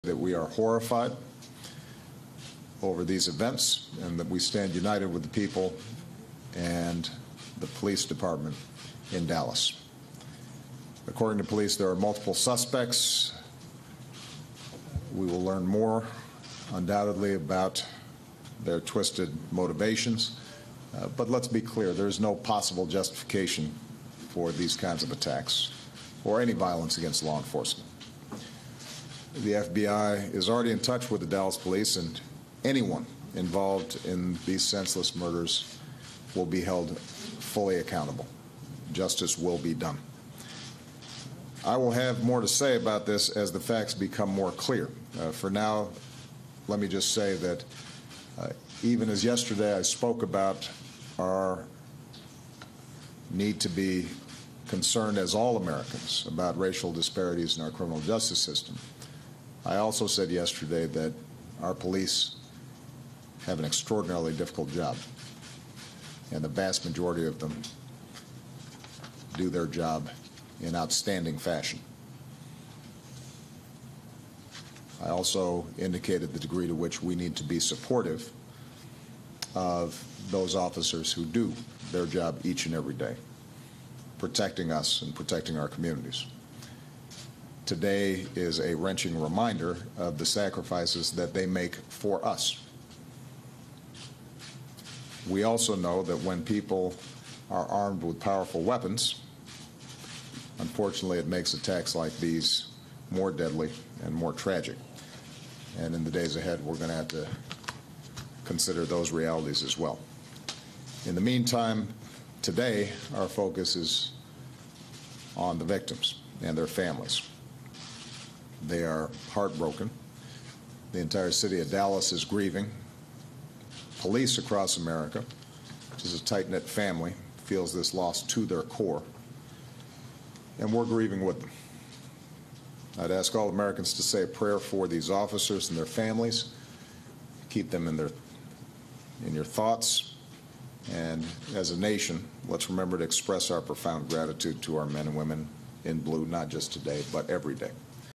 Obama: People Behind Dallas Attacks Will Be Brought to Justice
July 8 -- President Barack Obama speaks about the Dallas shootings that killed five Dallas police officers.